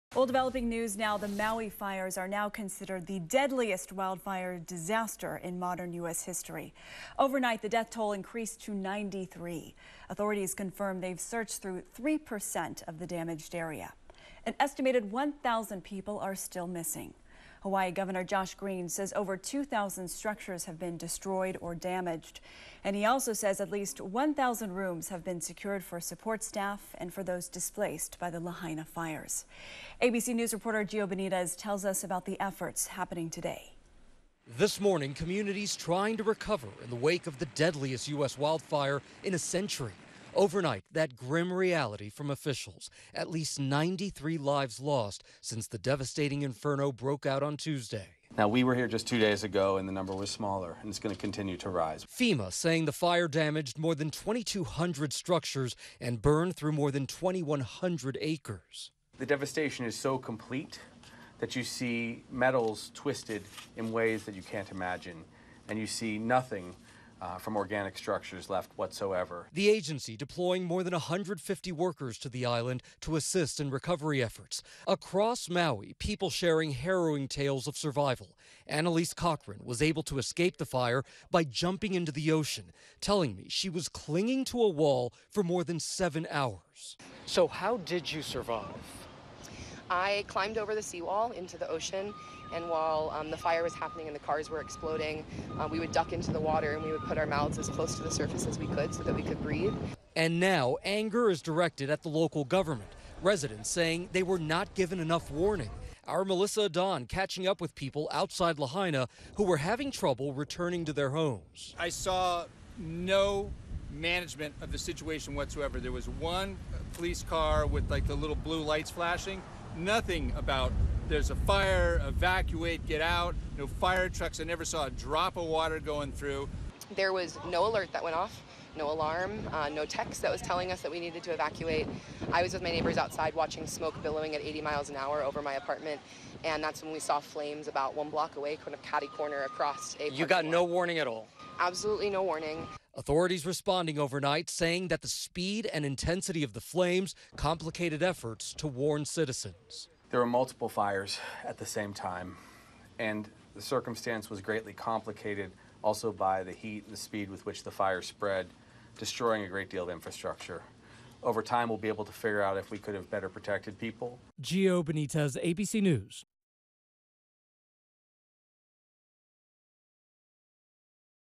Death toll climbs to 93 with 3% of destroyed area searched, officials say
As the death toll from a wildfire that razed a historic Maui town reached 93, authorities warned that the effort to find and identify the dead was still in its early stages.